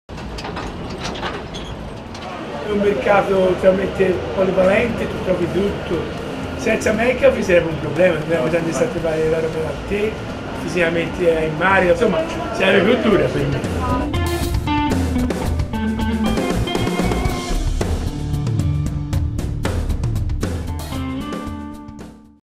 Alessandro - Promo - Mercafir è Firenze
Promo - Mercafir è Firenze - Alessandro (il tosto) racconta la sua storia, video completo su